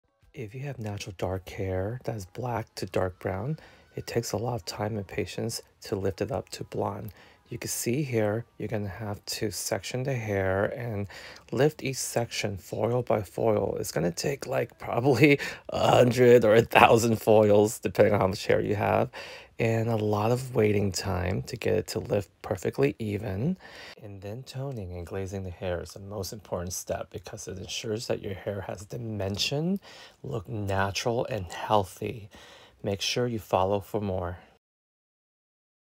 Black to Blonde in one session
The hardest struggle is doing a blonde retouch on a natural level 1-5 hair that’s grown out because you don’t want to overlap and cause breakage while you still want even lift scalp to ends
❤️ You can foil back to back  with 20vol or 30vol depending on the level you’re starting with and the result you want to achieve.
❤️I use my new @jrlusa heating halo processor.
❤️On my client I use GuyTang Big9 30vol 1:2 ratio with @guytang_mydentity Collagen Powder lift to a level 9.
❤️Glaze her hair dry with the following formula. All with Olaplex no1 any GuyTang Collagen powder.
1️⃣55g 10Ni Demi and 10g Naked 10 with 6vol on rootàge
2️⃣ 55g 10Ni Demi and 10g Naked 10 and 50g of CrystalClear with 6vol on mids and ends and process for 25mins.
❤️ Style using MyQuickBlow spray on mids and ends before drying the hair. Apply MyDefender heat protection spray before curling with iron and then break up curls and mist on MyStardust shine spray to add light shine and then spray MyPower hair spray for volume and hold of the style.
❤️All products are at Cosmoprof abd ArmstrongMccall and online.